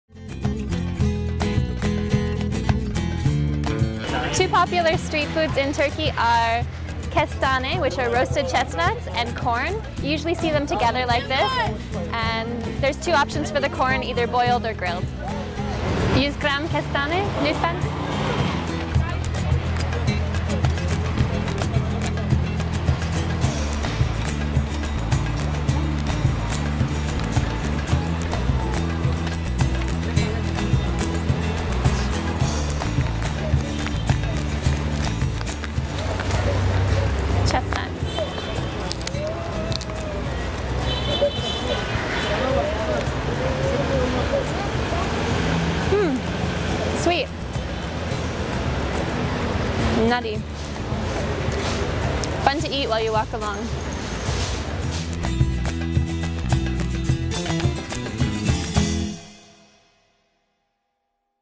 Chesnut & Corn - Kestane, Mısır
Chestnuts and corns are actually foods from opposite seasons,  chestnuts are winter fruits, and corns become ripe at the end of summer naturally. You can find them any season nowadays thanks to freezers, but we recommend eating them in season.
Chestnuts are a very valuable delicacy, they usually come from forests around Marmara, and sell rather expensive, 6 Liras for 100 grams at this vendor.
Corns are cheap, 1 or 2 Liras at most, but the varieties sold nowadays usually are extremely tasteless, "industrial" type corns, grown probably as animal food. Tasty corns are from heirloom seeds, and the difference is visually obvious : The grains are same size and orderly in industrial types, whereas it is just the opposite in the organic traditional corns. They are highly recommended.